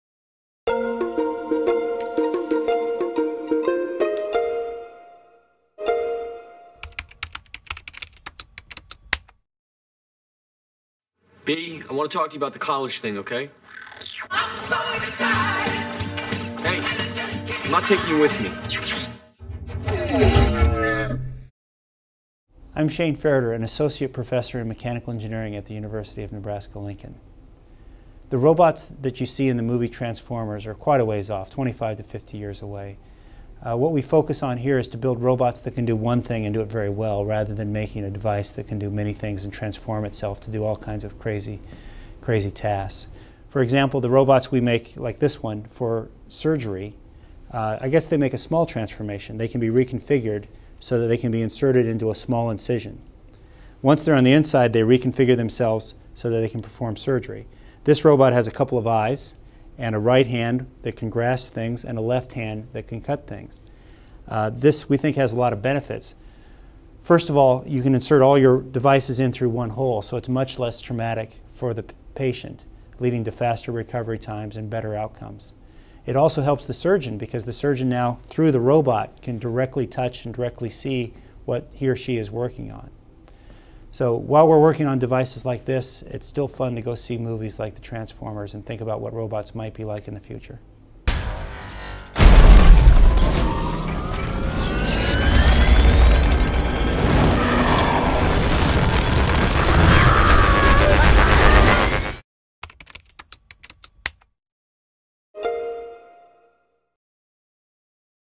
N The Know - DO robots like the "Transformers" already exist?
Featuring UNL expert Shane Farritor, an Associate Professor of Mechanical Engineering at the University of Nebraska-Lincoln, who shares his unique insight on the current development of robots and answers the question: "Do robots like the "Transformers" already exist?".